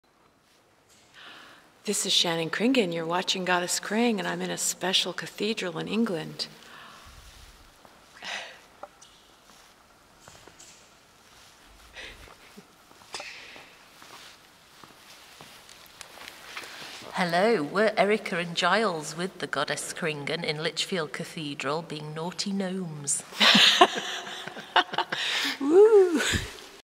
Goddess KRING Lichfield Cathedral England
Goddess KRING and friends at the Lichfield Cathedral England. 0142